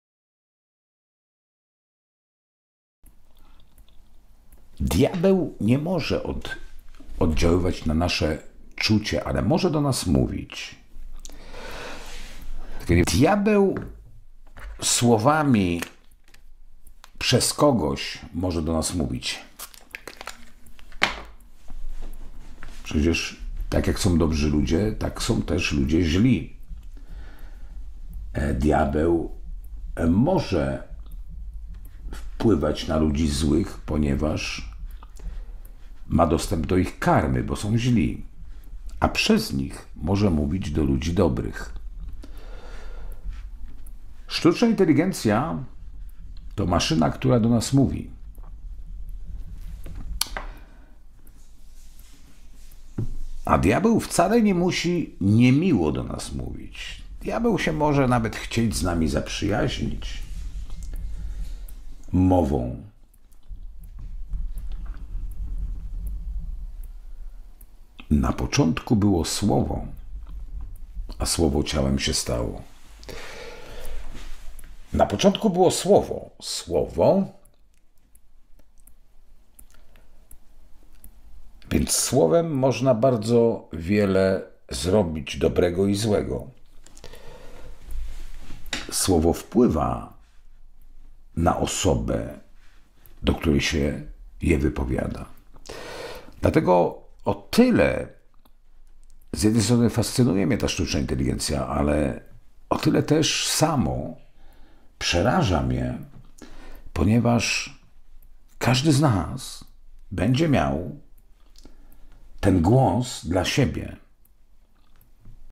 AI and the Devil. English CC. Polish clairvoyant Krzysztof Jackowski's predictions
To see the English translation, click on the CC icon (on the computer) or click on the video screen, click on the cogwheel symbol and choose English subtitles (on the phone).